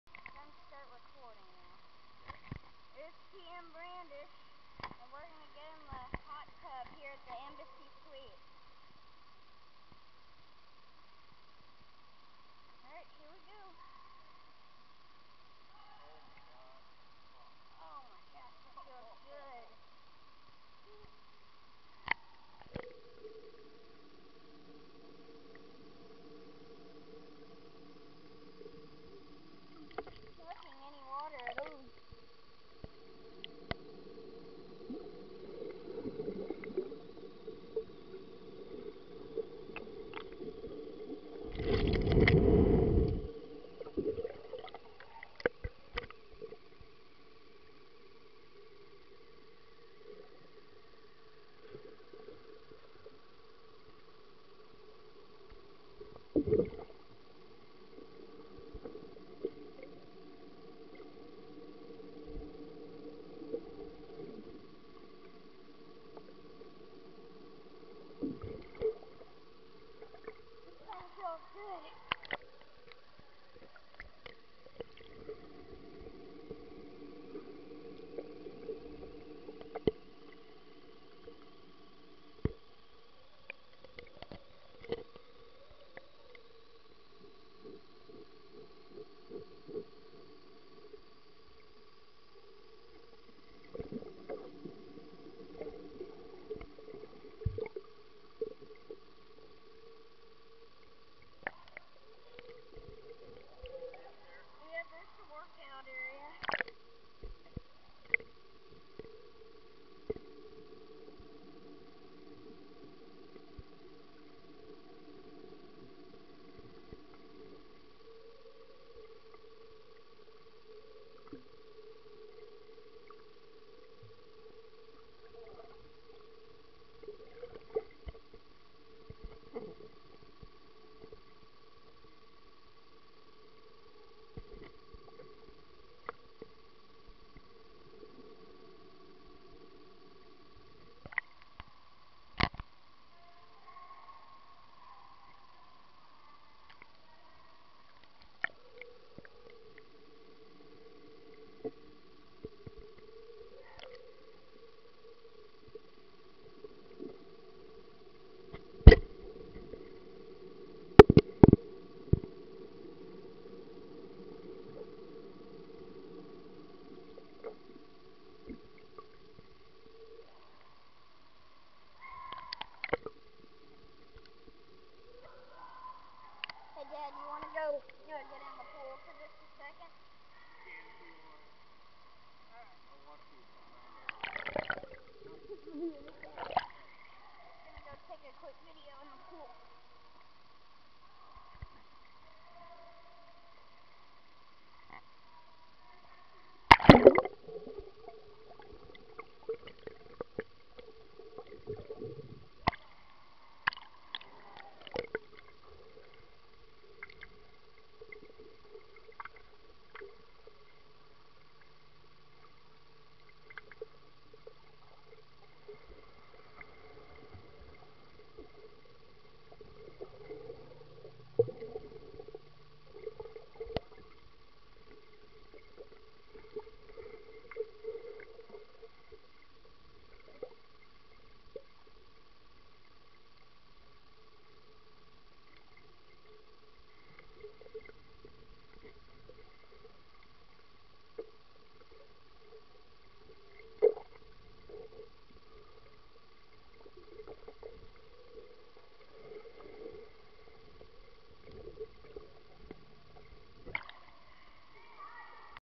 pool review: 6 VGB complient pool drains @ The Embassy Suites pool Frisco TX
in this video we actuly went under water and got an upclose look.